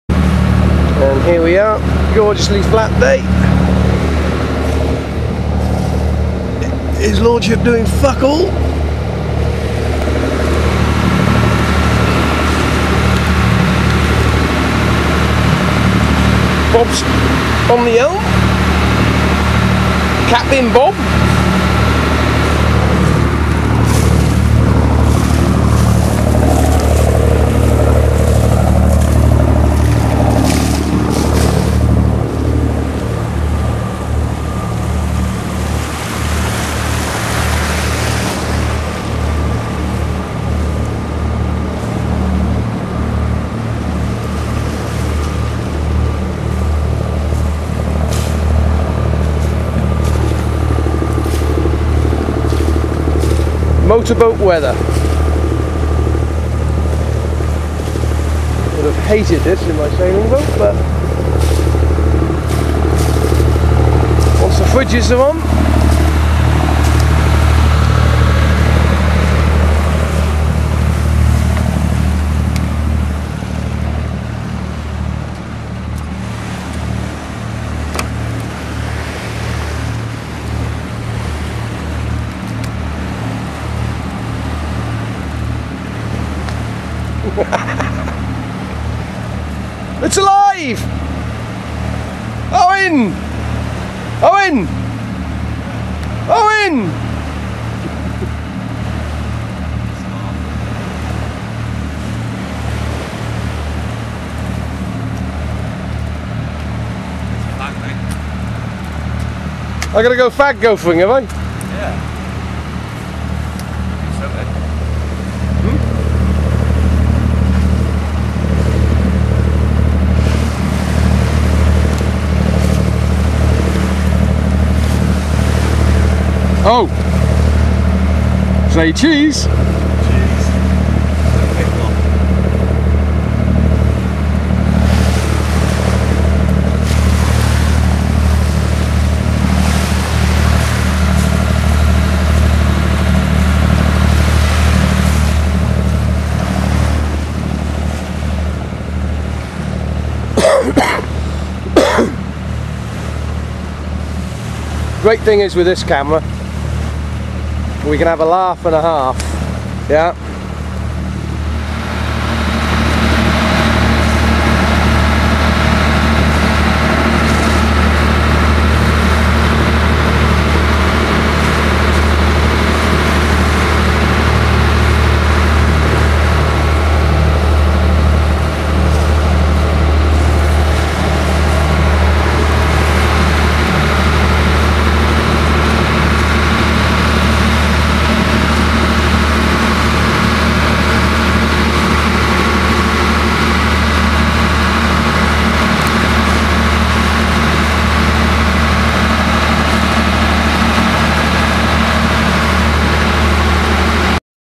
Owen Williams from caerwent first sea trip
Owen Williams first sea trip on silver dollar